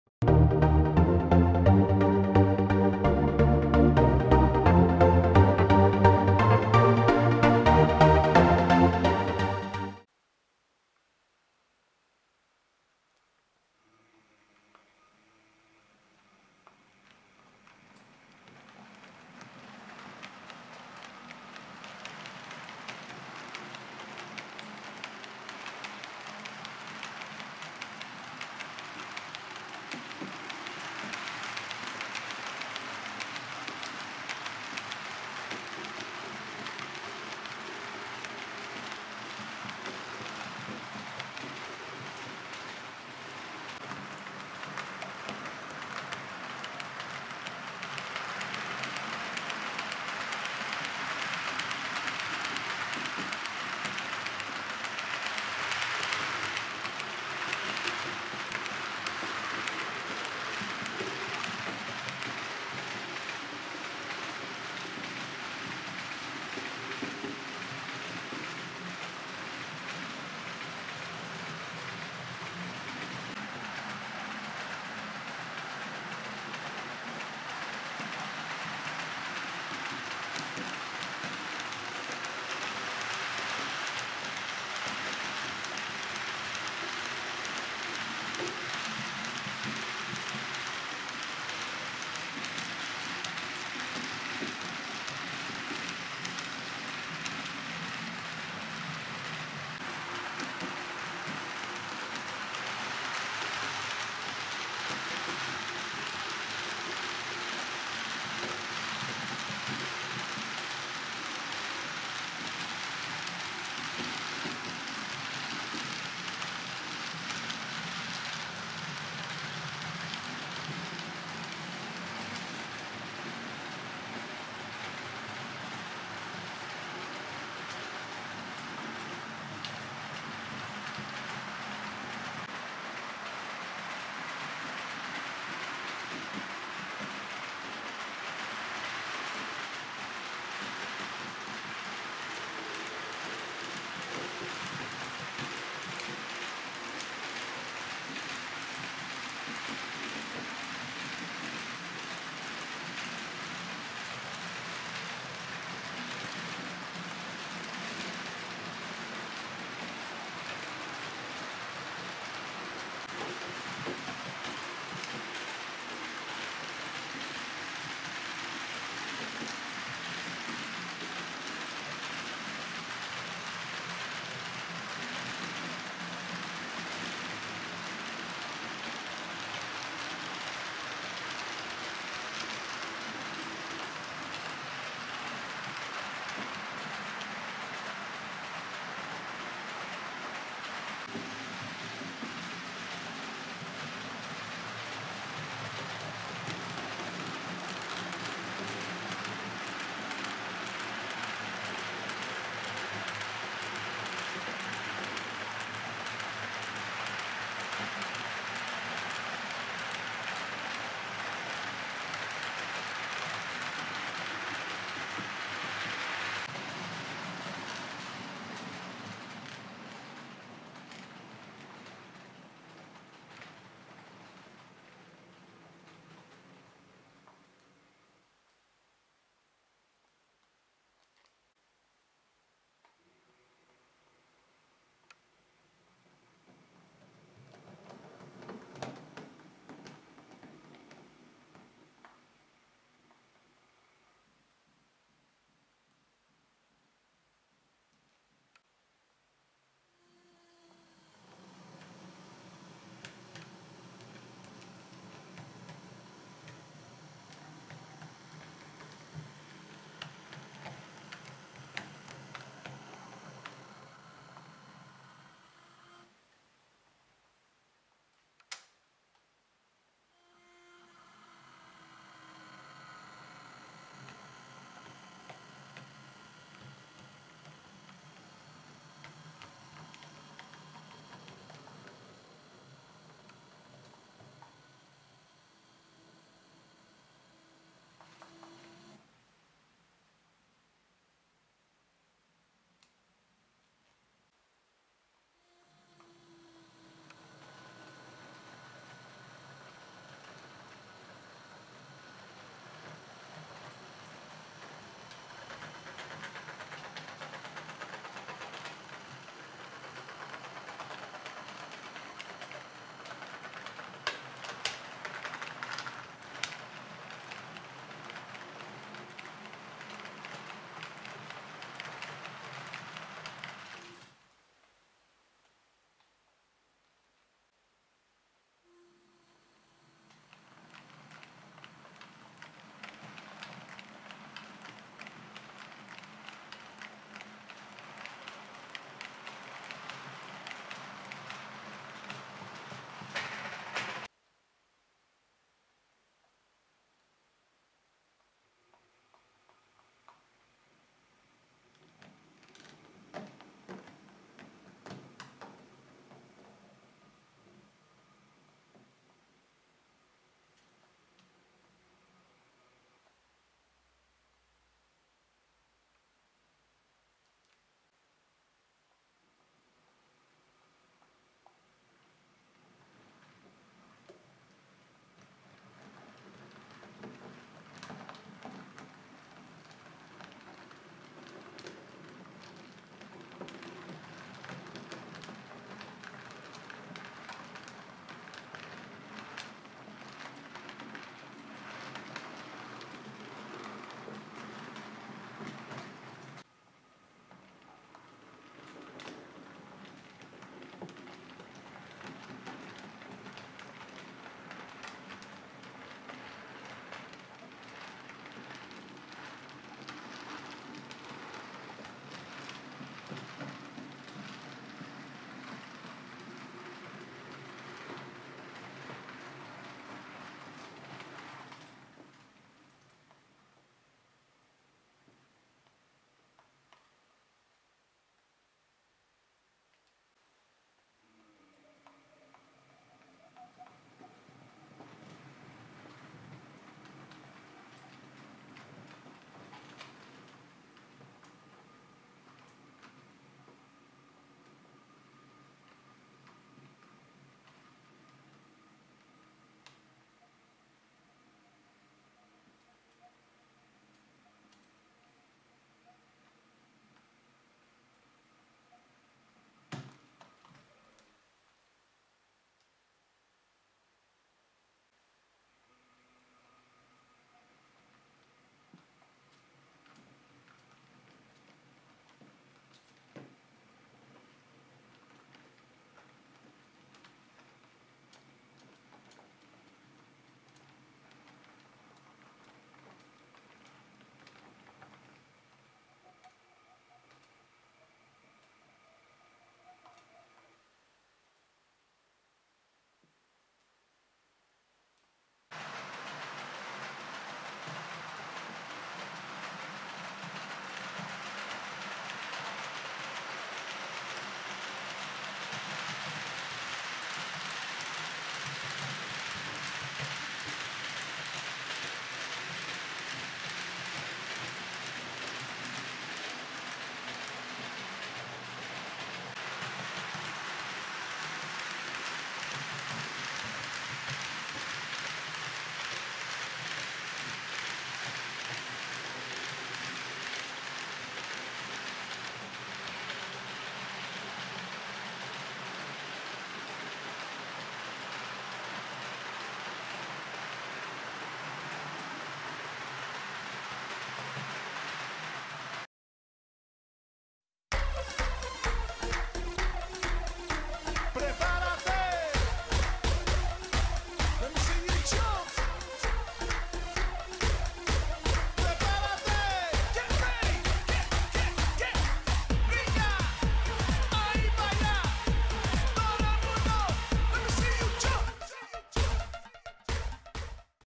Na życzenie: Manewry lokomotyw Roco z węglarkami i cysternami BP
Film przygotowany dla kolegi "Kaprix !".

Miłego oglądania!